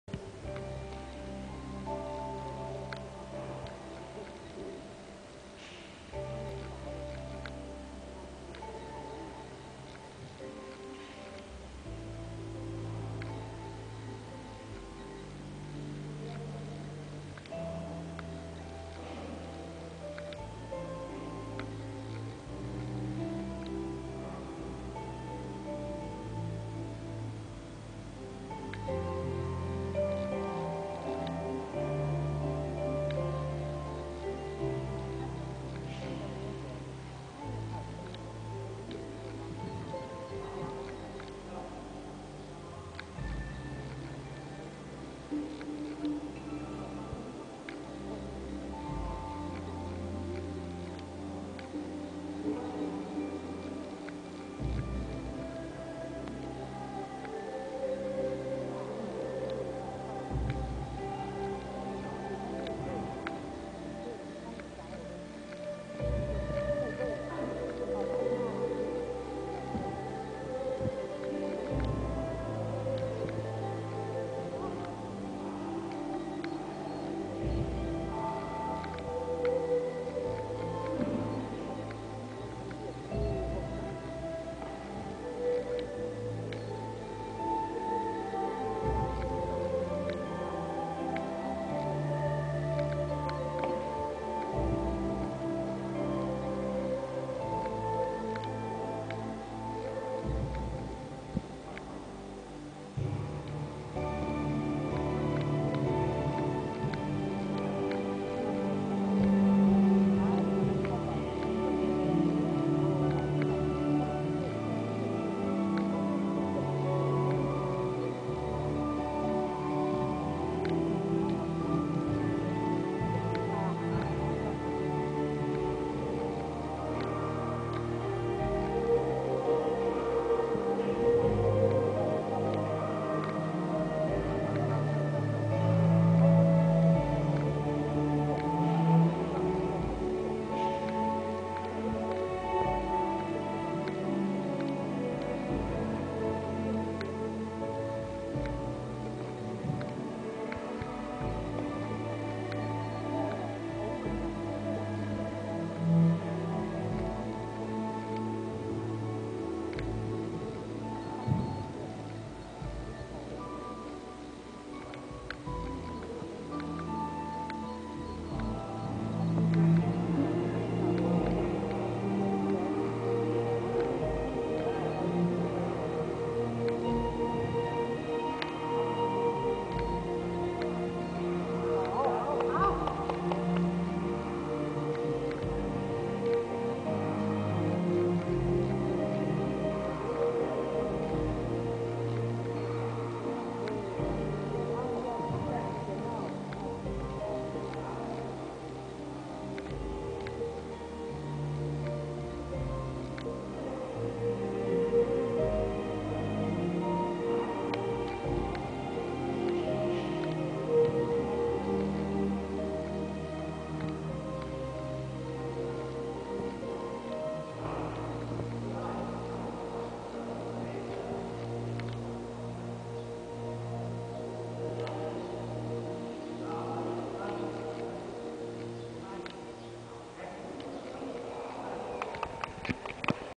Free style taijiquan
I recorded this free taijiquan performance with a simple camera, so the quality is not that good.
The performer is  周菁 （zhou jing） and her coaches name is 赵勇（zhao yong）. This recording was made a the Hubei provincial sports school where champions in all sports disciplines are trained. Impressive stuff.
周菁 got first price in last years championship in Putian in Fujian province.